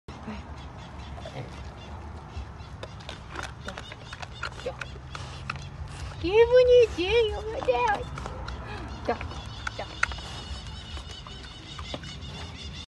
Hungry pig eats corn 🇧🇷 Brazil
Hungry pig eats corn in Brazil.